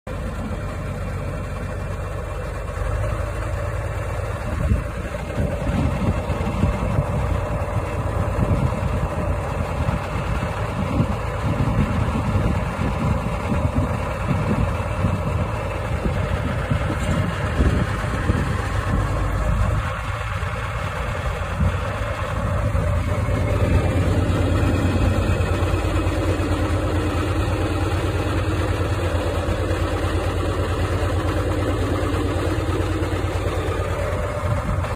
1974 JOHN DEERE 4030 For Sale
1974 John Deere 4030- 5,614 hours showing, quad range, 2 hydraulic remotes, 3 point, 540/1000 PTO, 420/80 R34 tires. Has been on the same farm since 1996.